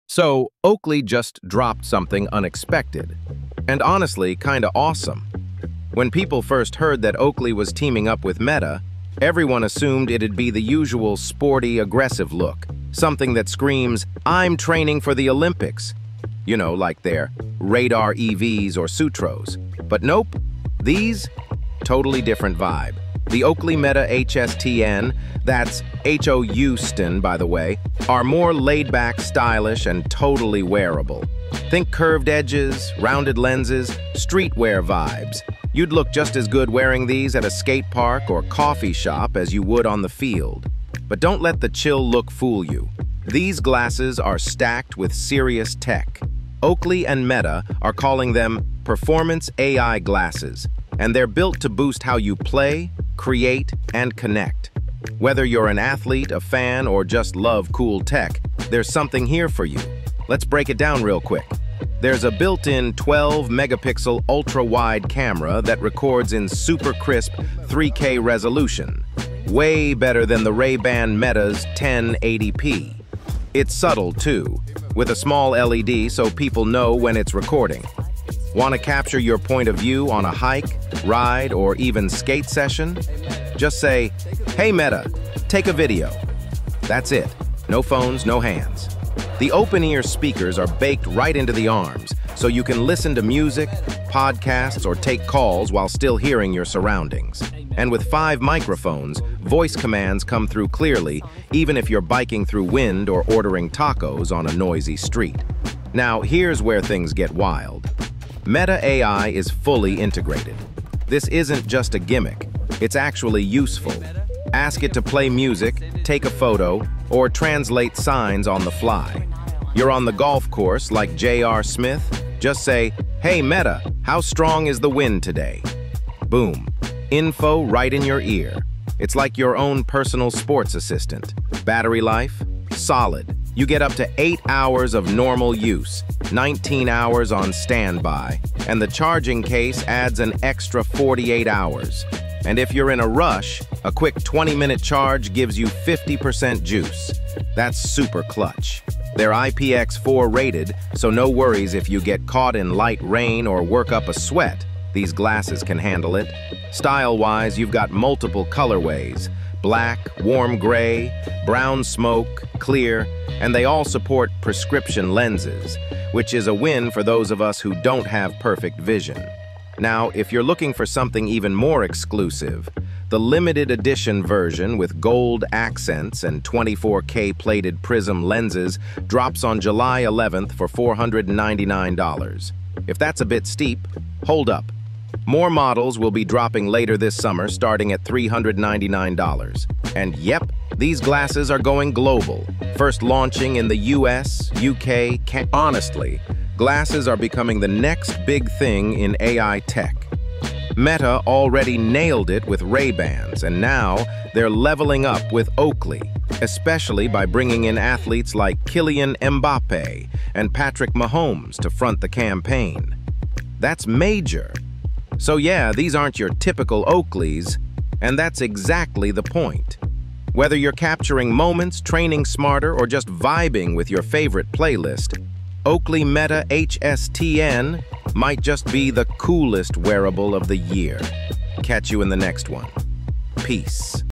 Oakley Meta HSTN – Next-Gen Smart Glasses with Meta AI!
Oakley Meta HSTN Review – Next-Gen Smart Glasses with Meta AI!

Oakley and Meta just teamed up to launch their first Performance AI Glasses — the Oakley Meta HSTN. These aren't your typical Oakleys. In this video, we take a casual yet detailed look at what makes these smart glasses stand out, from their sleek, streetwear-inspired design to the powerful tech packed inside.

We’ll cover:

3K ultra-wide camera
Built-in Meta AI assistant
Open-ear speakers + 5 microphones
8-hour battery life (plus 48 hours with case)
Water resistance (IPX4)
Style options, prescription lens support & more

Whether you're an athlete, a creator, or just someone who loves cool wearable tech, these glasses offer a fresh take on what smart eyewear can be.

🔥 Limited Edition with 24K Plated Prizm Lenses drops July 11 ($499)
💡 More models launching later this summer for $399